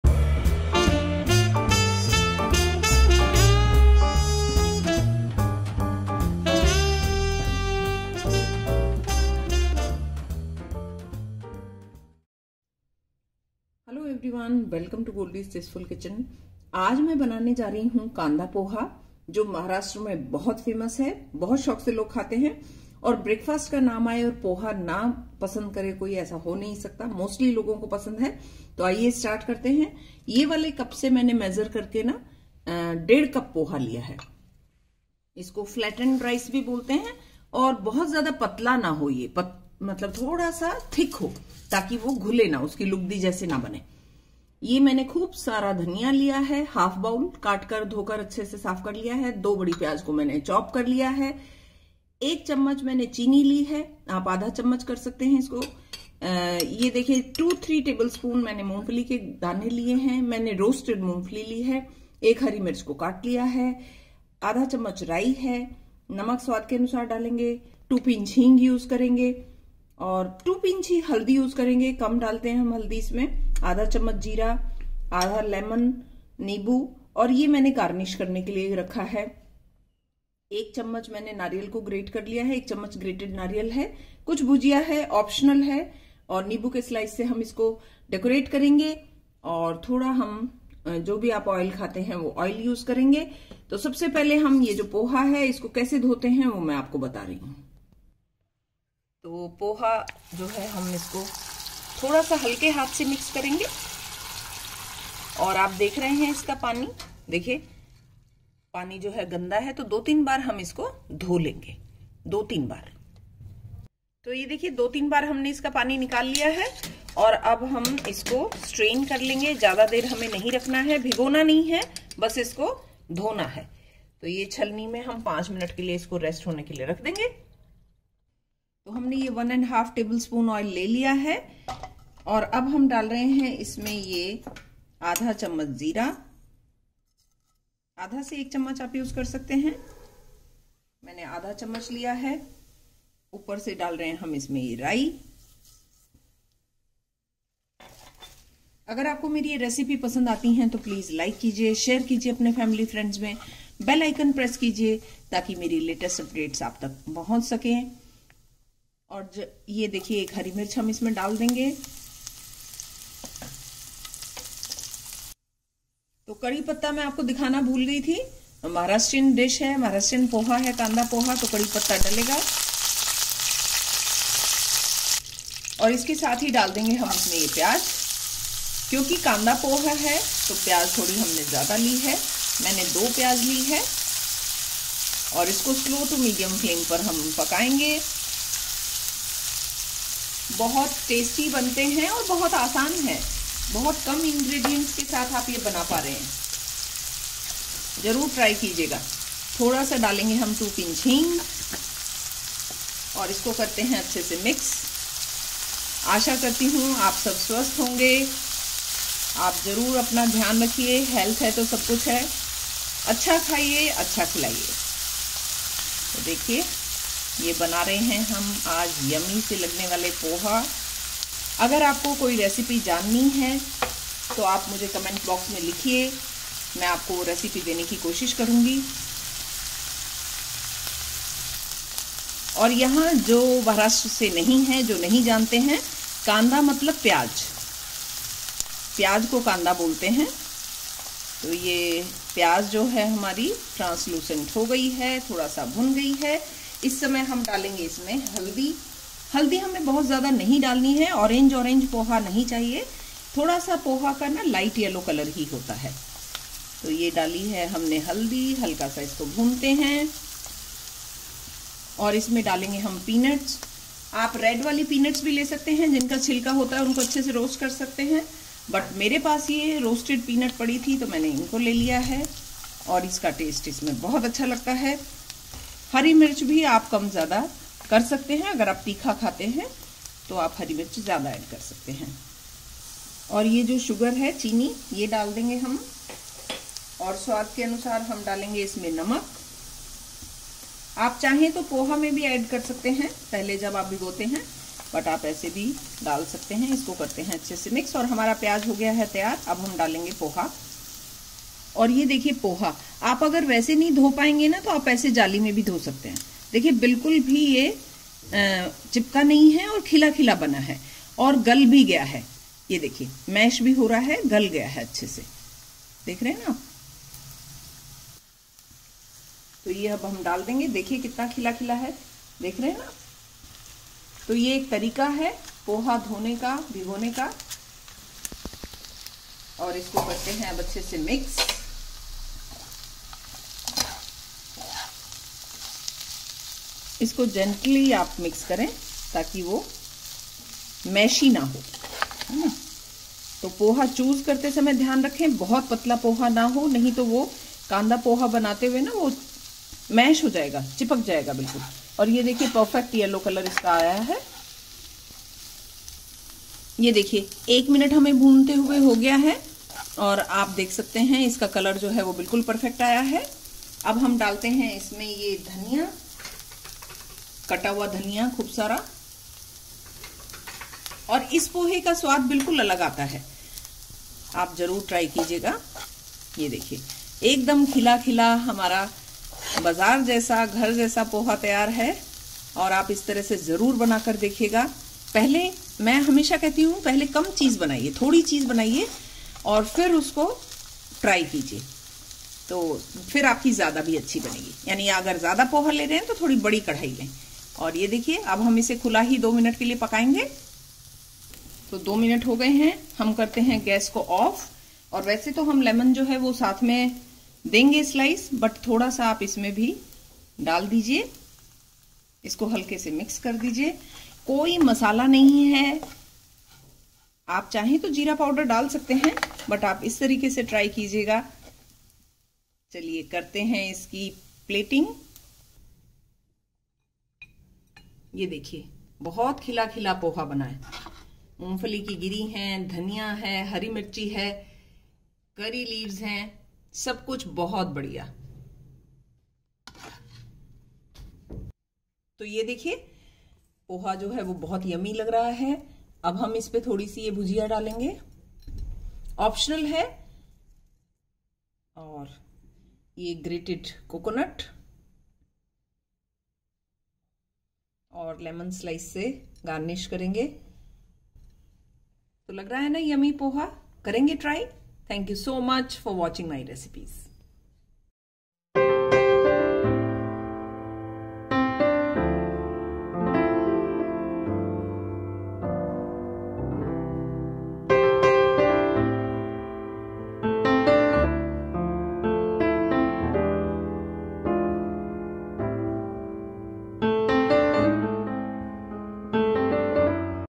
Mumbai Style Poha Recipe || कांदा पोहा बनाने का आसान तरीका
INGREDIENTS:
1) Poha (Flattened rice) - 1.5 cup;
2) Onion (finely chopped) - 2 big;
3) Oil - 1.5 tbsp;
4) Coriander leaves (chopped) - 2 tbsp;
5) Sugar - 1 tsp;
6) Roasted peanuts - 2 tbsp;
7) Green chilli (chopped) - 1;
8) Rai - 1 tsp;
9) Salt - as per taste;
10) Heeng - 2 pinch;
11) Haldi - 2 pinch;
12) Lemon - half;
13) Cumin seeds - 1/2 tsp;

FOR GARNISHING:
14) Grated coconut - 1 tbsp;
15) Bhujia - 2 tbsp;
16) Curry Leaves - 8 to 10;

How to make Kanda poha, Easy and quick way to make poha, easy and quick poha recipe, Maharashtrian poha recipe